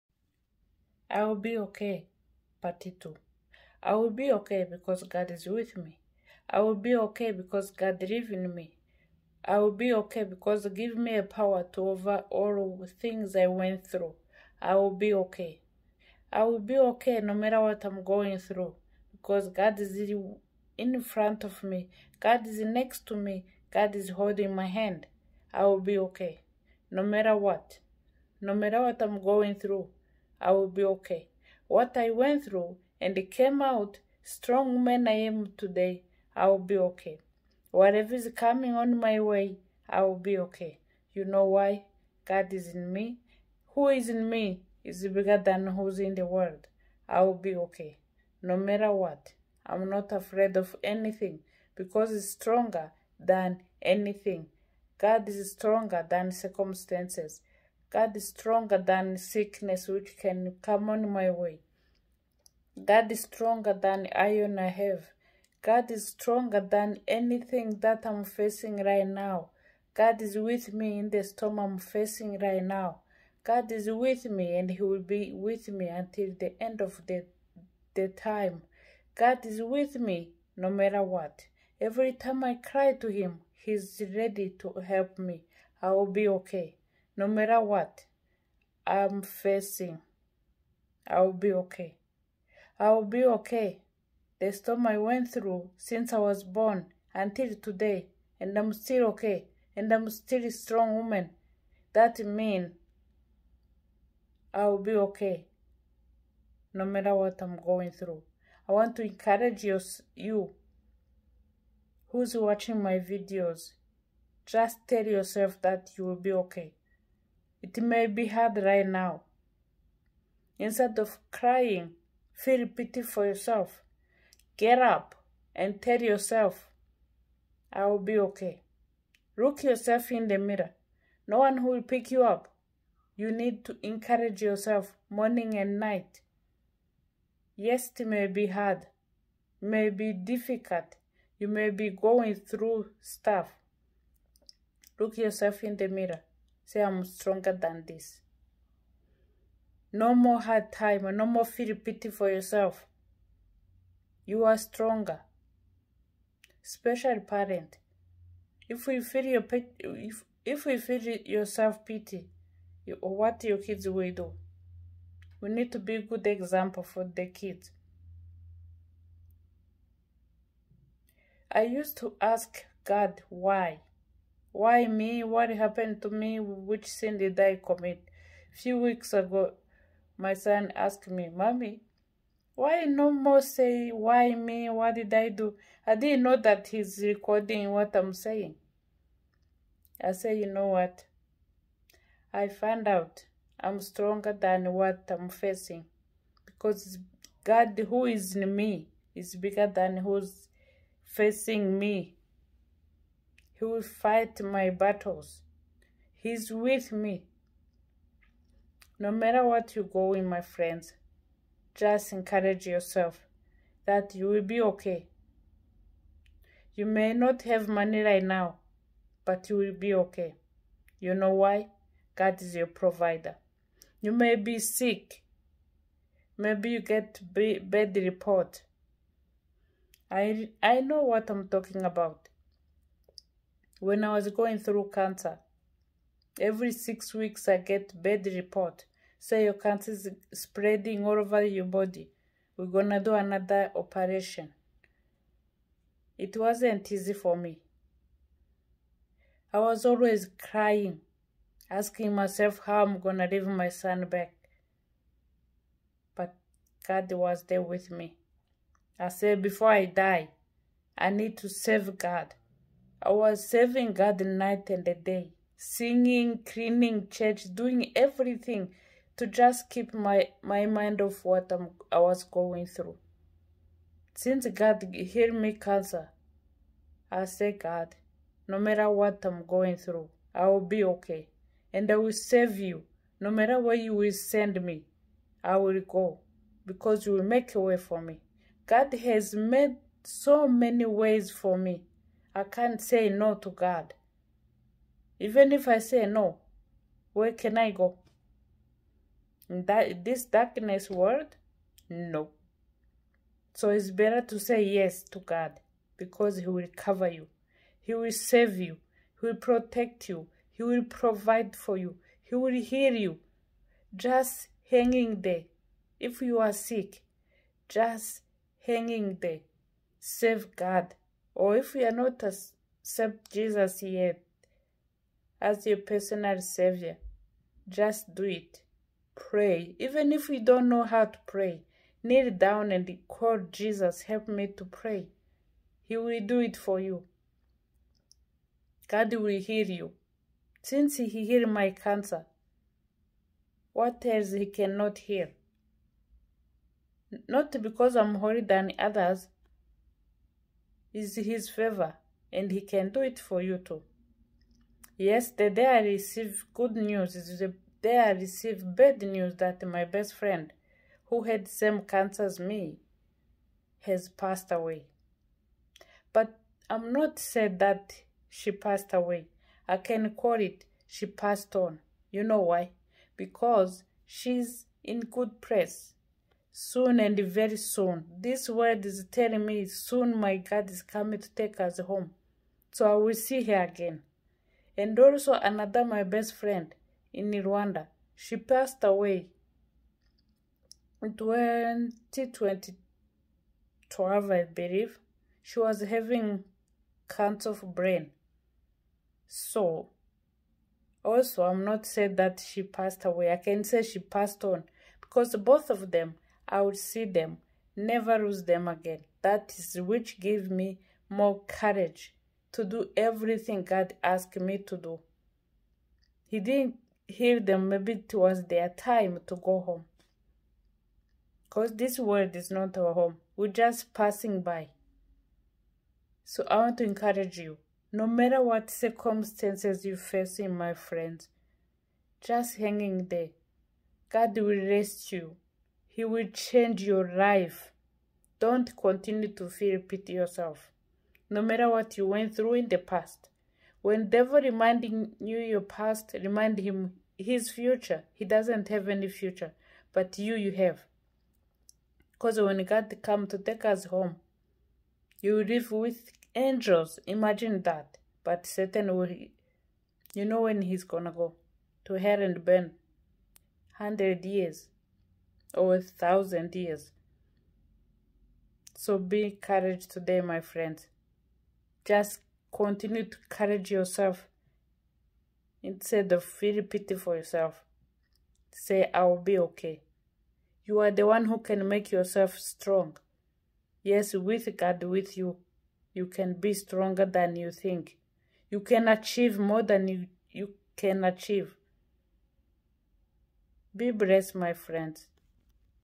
I will be OK 👍 part 2
when you know who is in you and whom you serve there's nothing can and will scare you. and when you look back what went through and it didn't destroy you that is another reason to say that you will be OK.